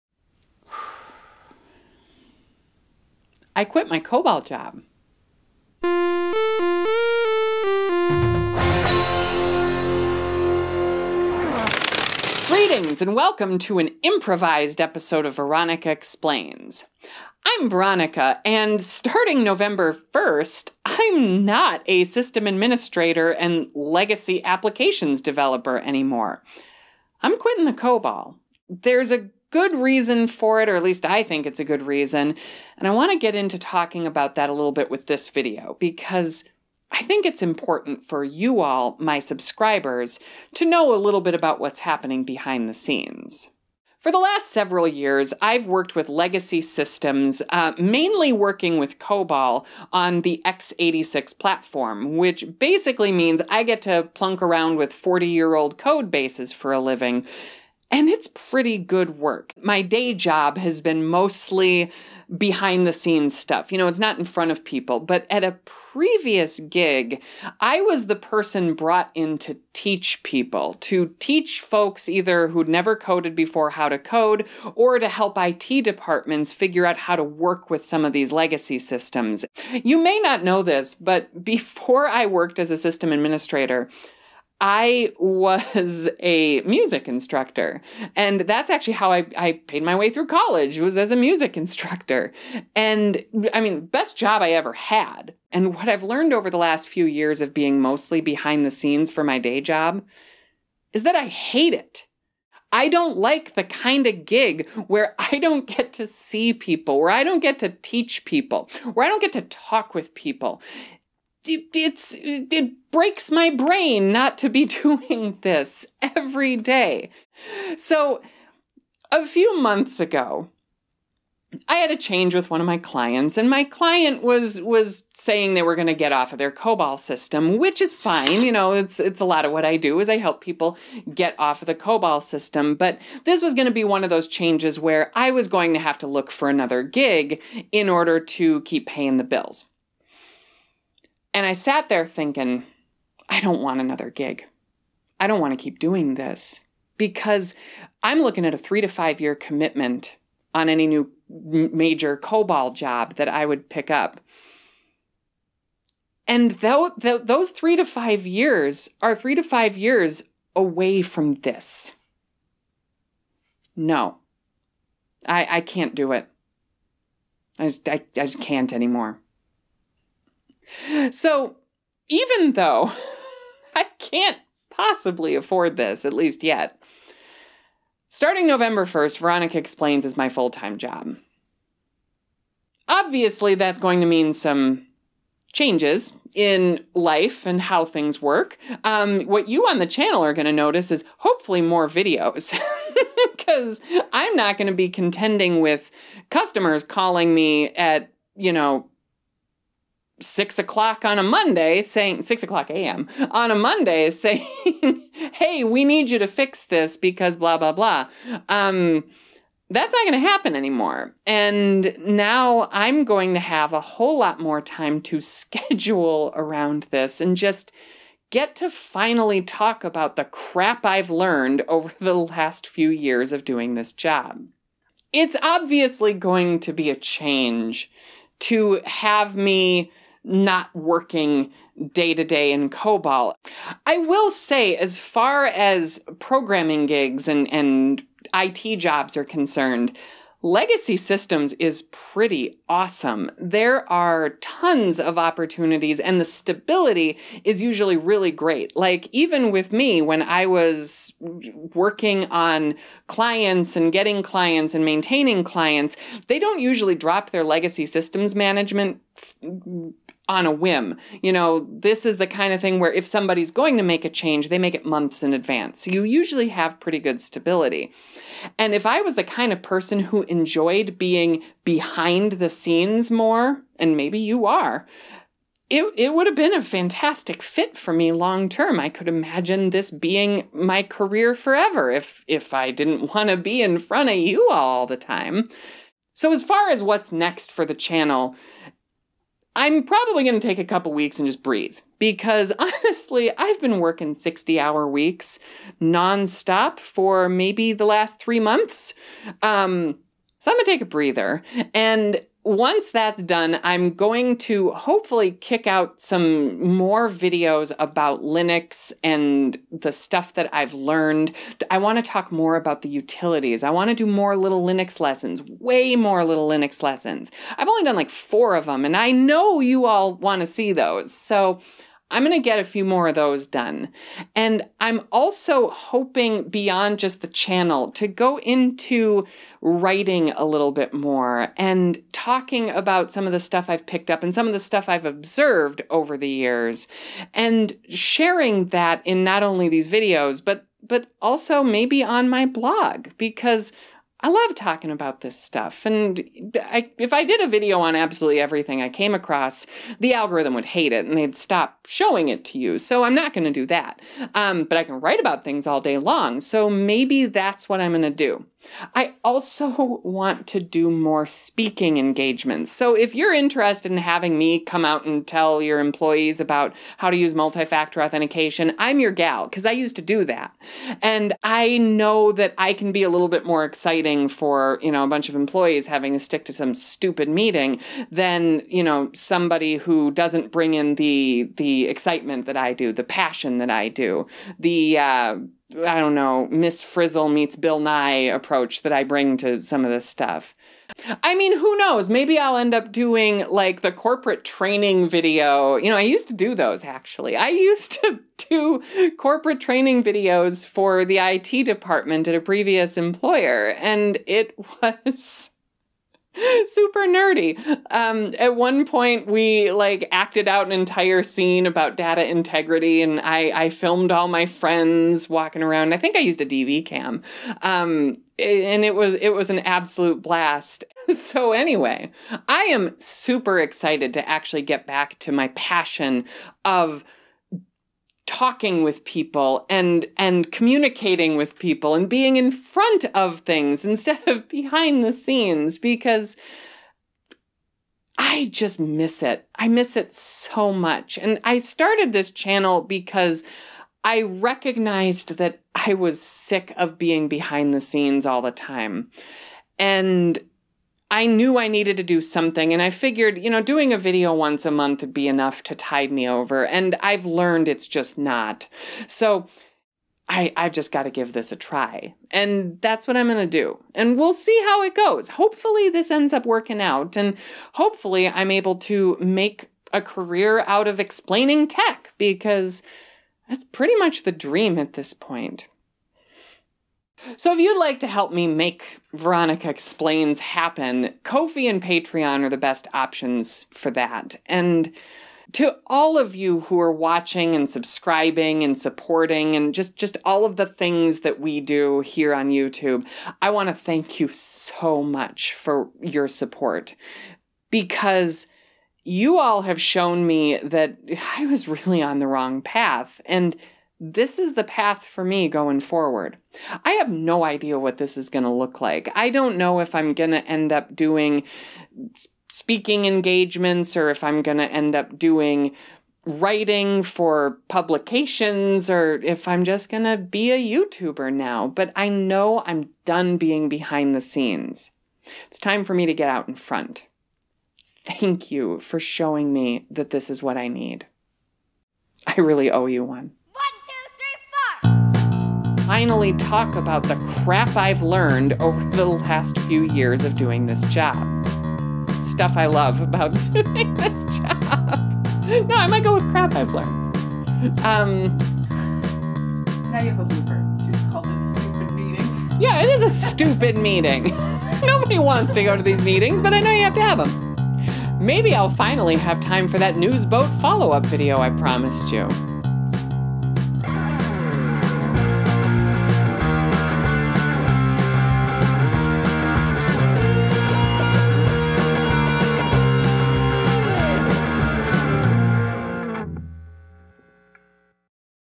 IT career changes: here's why I just quit my job.
In today's episode, I talk about why I just quit my job as a system administrator and COBOL developer.

It's OK to be ready for change.

Also, I used a picture of Miss Frizzle from Magic School Bus, which is owned by Scholastic. I also used a photo of Bill Nye that was posted on his website. Neither endorsed this video nor were consulted.

Chapters:
0:00 I quit
0:43 Career background
1:35 Realizations
3:53 Is COBOL OK?
4:46 What's next for the channel?